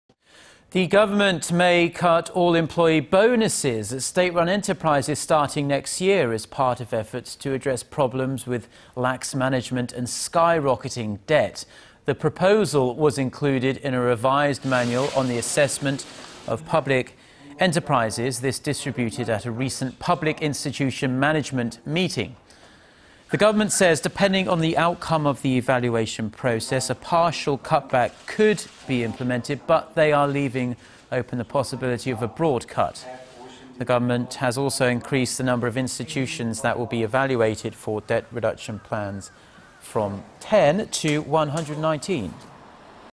Government may cut all bonuses of state-run enterprise's employees
The government may cut all employee bonuses at state-run enterprises starting next year, as part of efforts to address problems with lax management and skyrocketing debt.
The proposal was included in a revised manual on the assessment of public enterprises distributed at a recent Public Institution Management Meeting.
The government says, depending on the outcome of the evaluation process, a partial cutback could be implemented, but they are leaving open the possibility of a broad cut.
The government has also increased the number of institutions that will be evaluated for debt reduction plans from 10 to 1-hundred-19.